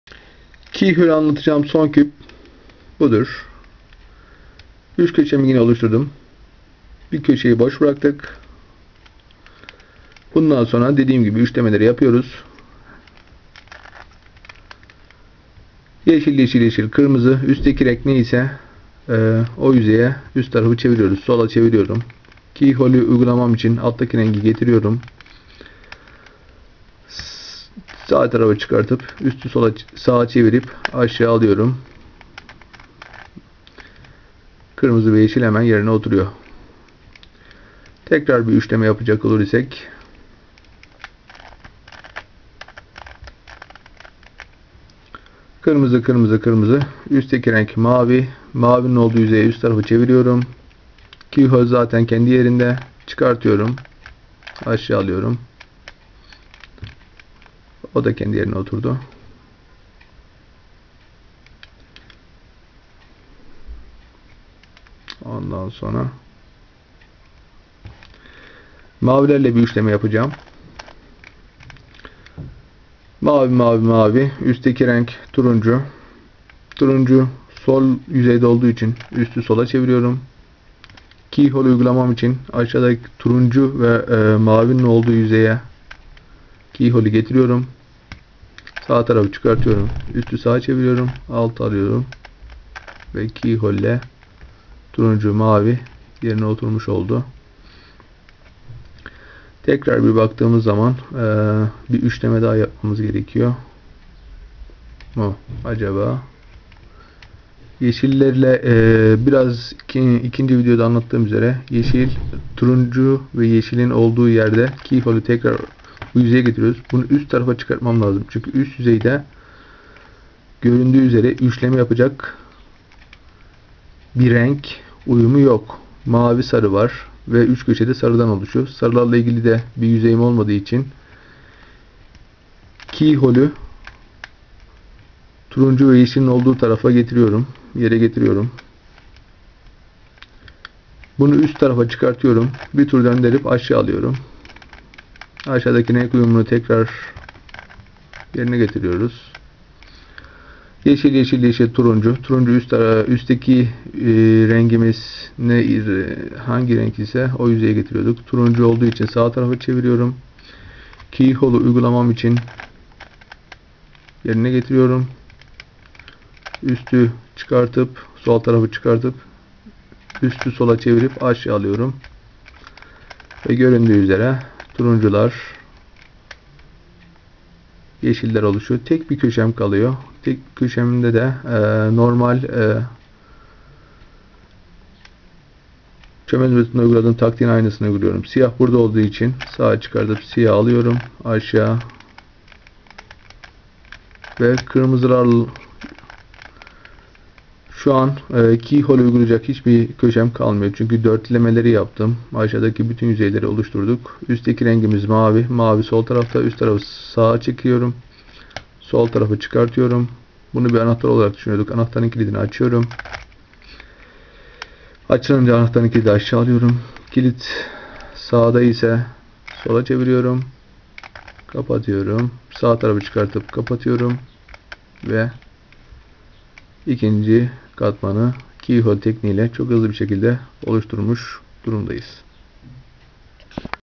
KEYHOLE{Metodu - Tekniği} Rubik küp uygulamalı anlatım part3
ilk katmanı oluşturulmuş ve tek köşesi KEYHOLE için bırakılmış küp ile 2.nci katmanı tamamlama videosu (SyF) kısa şekilde anlatım yaparak genel itibari ile uygulamalara yer vererek  Keyhole tekniğini anlatma